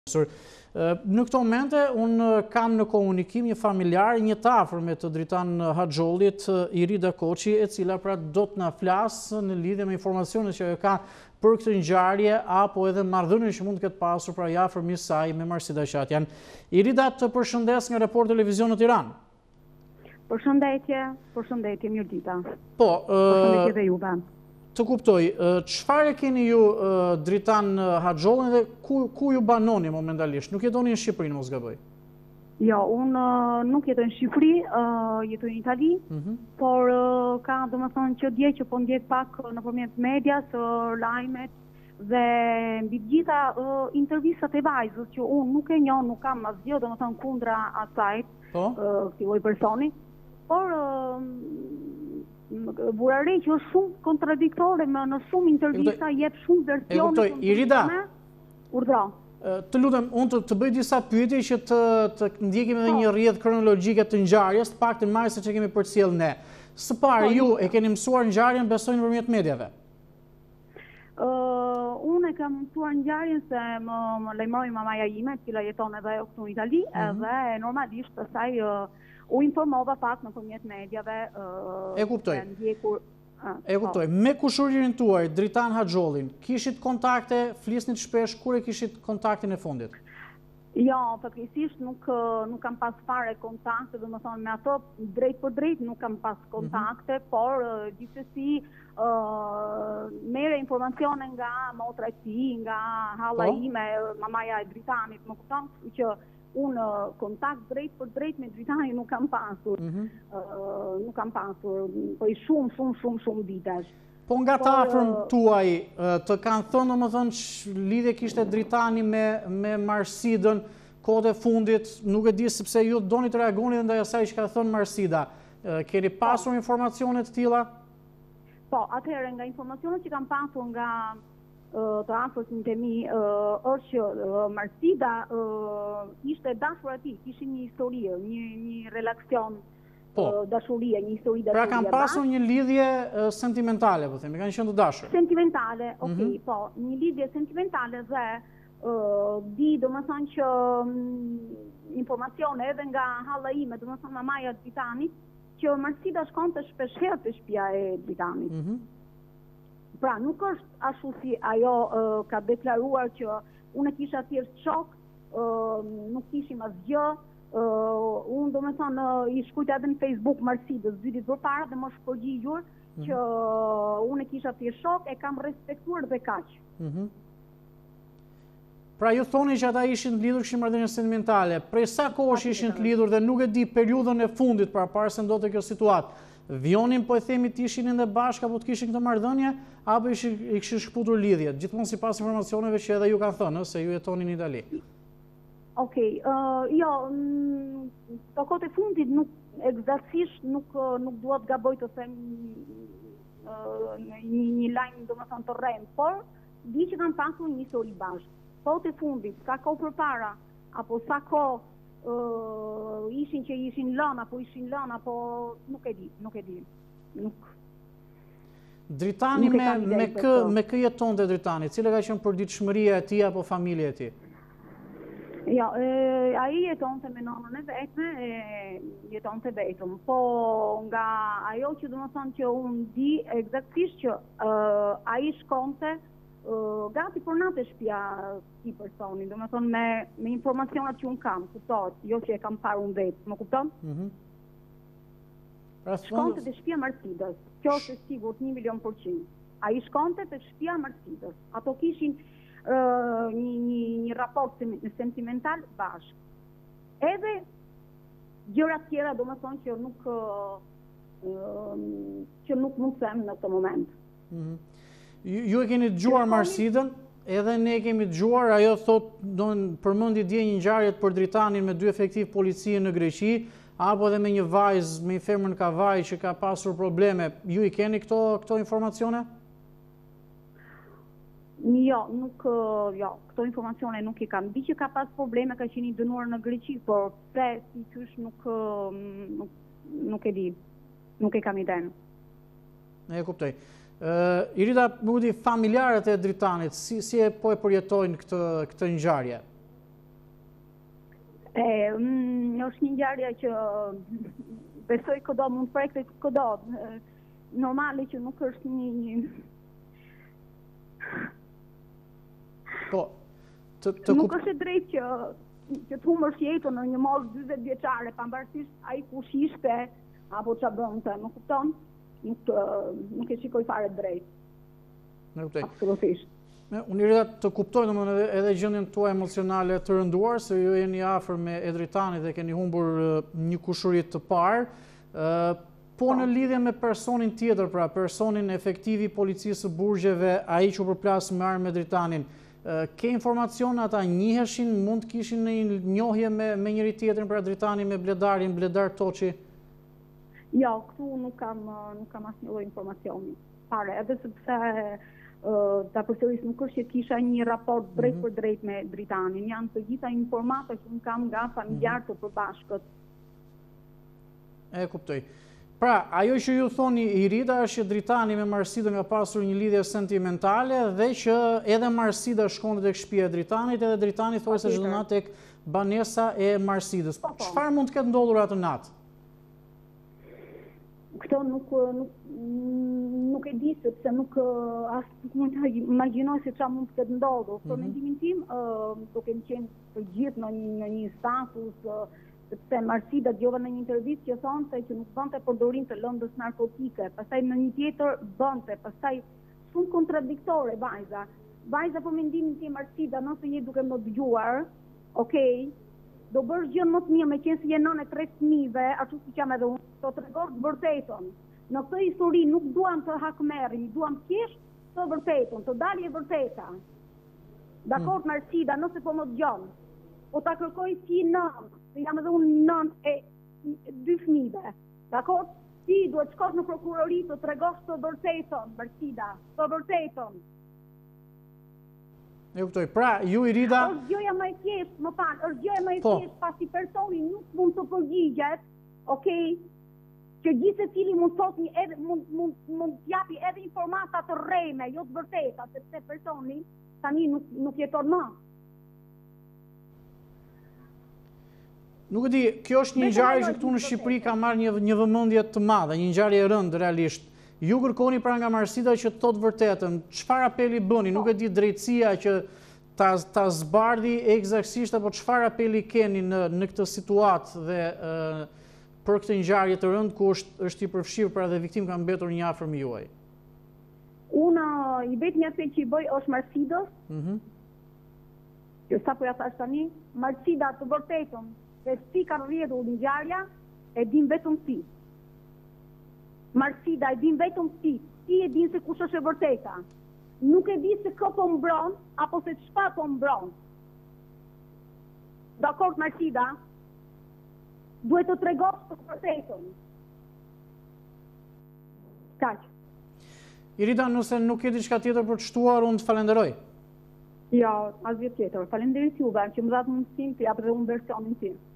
Ekskluzive/ Flet për Report Tv kushërira e viktimës në Kavajë: S'duam hakmarrje!
Irida Koçi, kushërira e parë e Dritan Haxhollit që mbeti i vdekur nga përplasja me armë në Kavajë dy ditë më parë, në një lidhje telefonike për Report Tv nga Italia ku jeton  ka një apel për Marsida Qatjan.